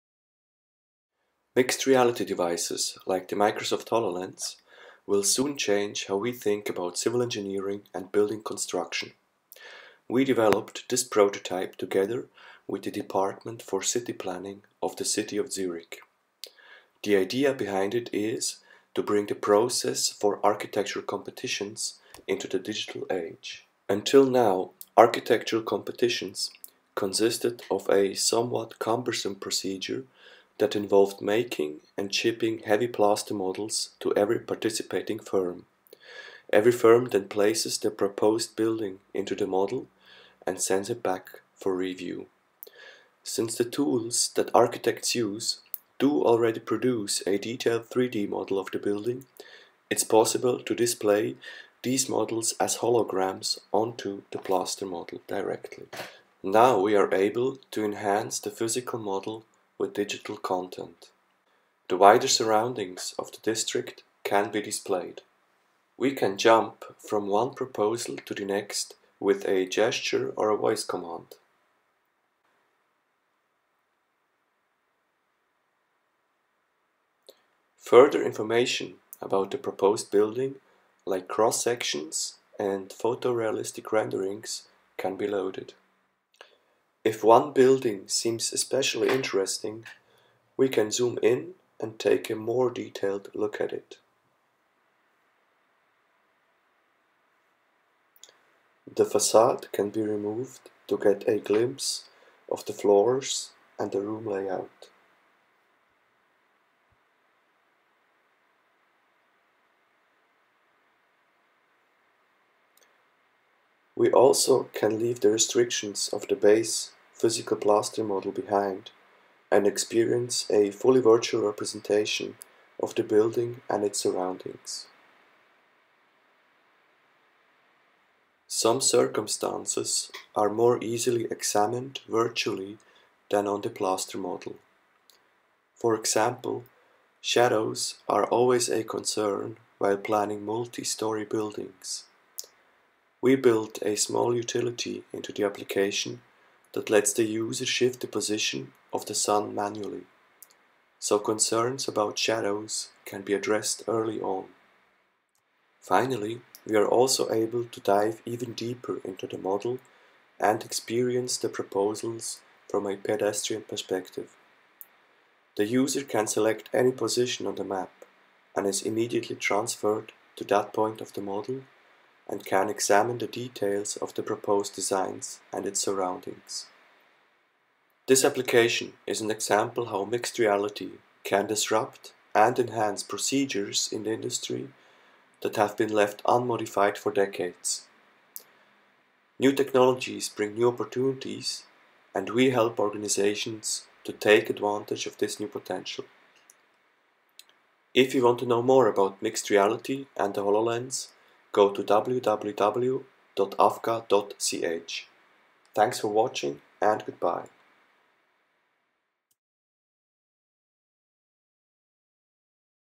City Planning with HoloLens - Mixed reality showcase
This application displays 3D buildings in mixed reality onto a plaster model. It simplifies and enhances the process in architectural competitions.

Follow us for more HoloLens 2 and Mixed Reality content!